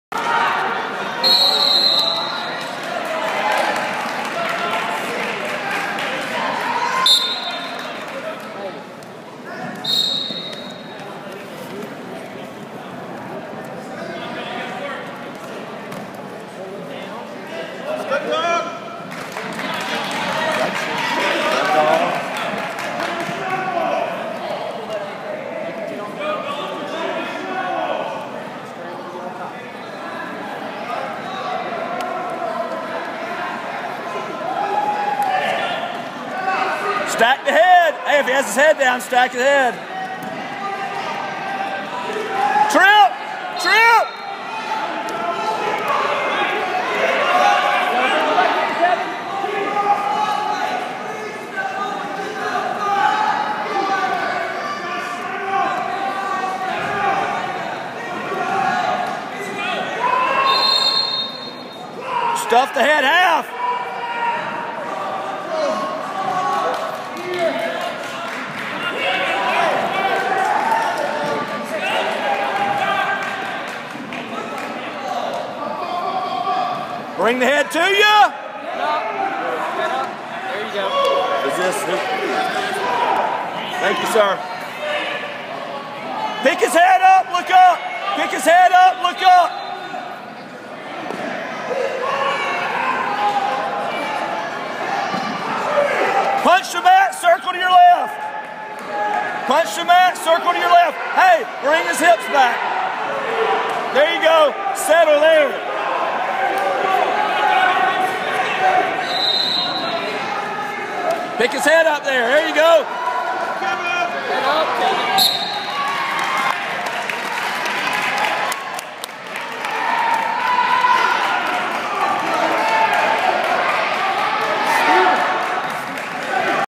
Ram Tough Duals First Match of the 2012-2013 Season vs. Luella High School 152 lbs.
First match of Kevin's Senior Year!